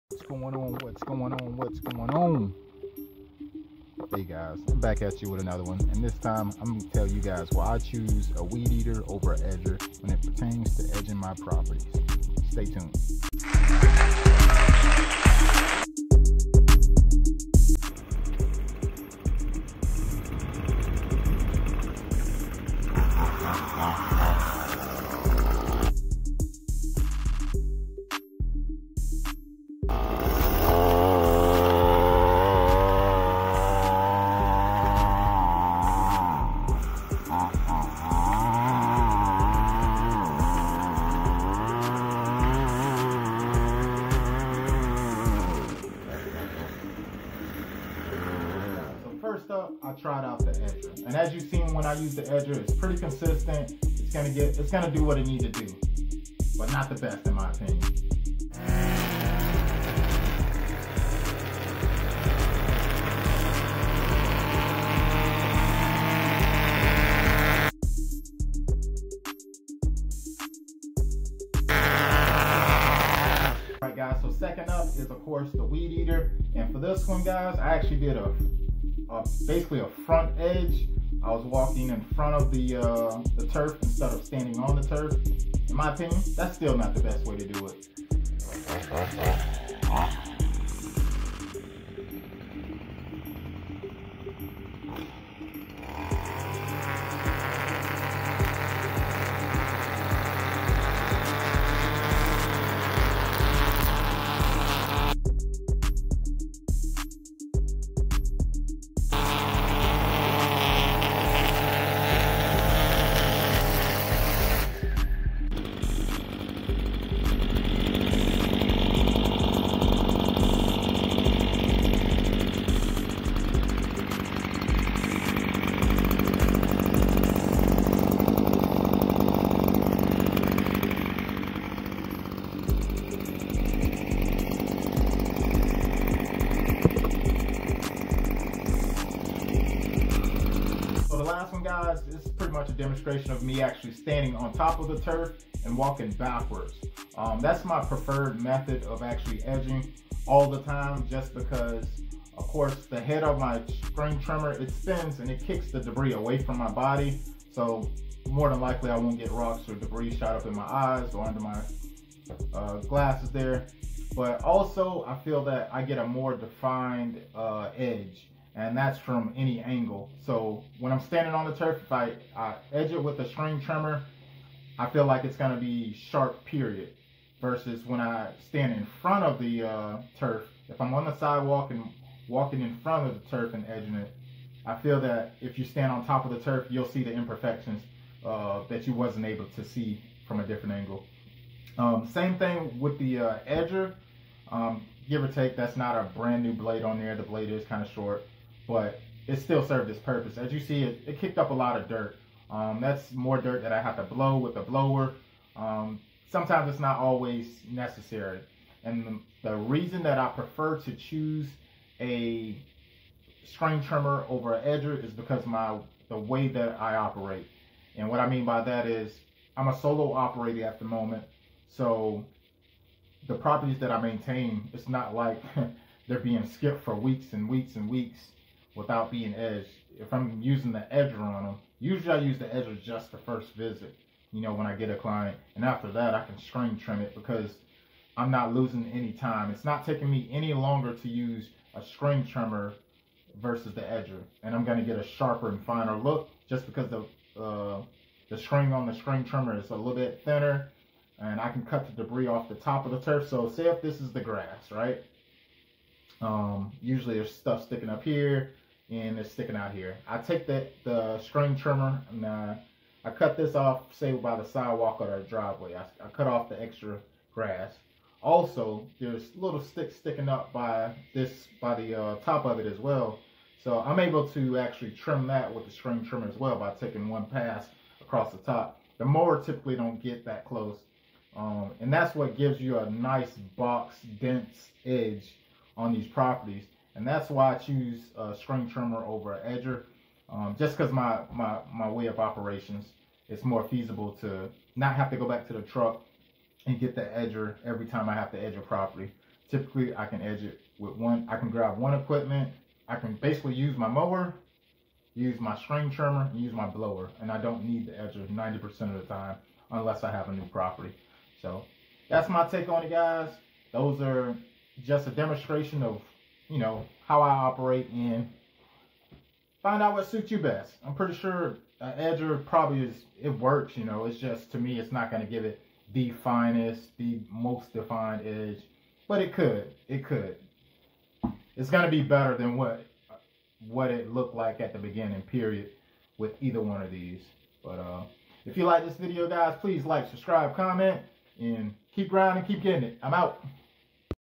String Trimmer or Edger ? 🤔| Lawn Care | Landscaping
IMO i feel that is best to use a piece of equipment that is versatile. Enjoy the video!

I have thought long & hard about what works for me in my business. I have came to a conclusion that i prefer a string trimmer/ weed eater over an edger. I explain why in the video.